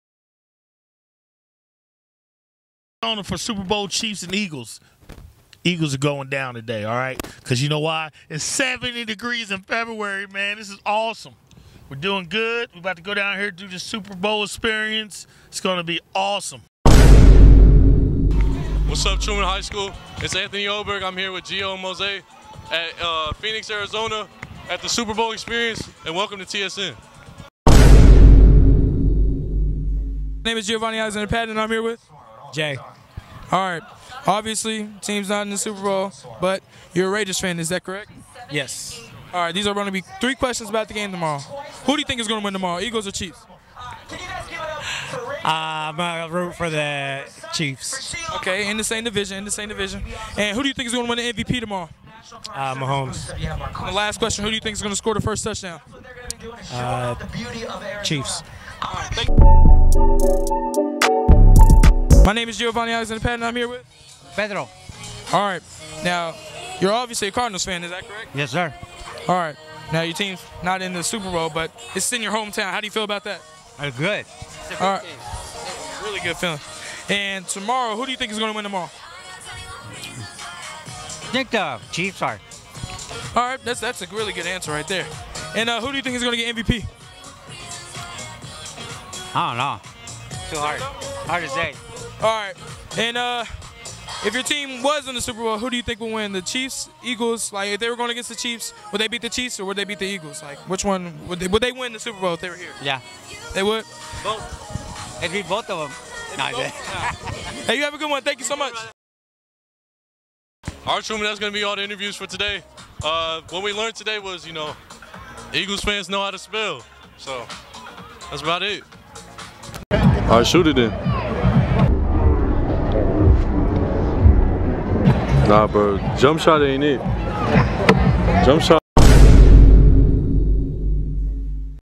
Interviews from the Super Bowl
A couple members of the THS Media staff traveled to the Super Bowl to take in the sights and sounds of the Super Bowl atmosphere.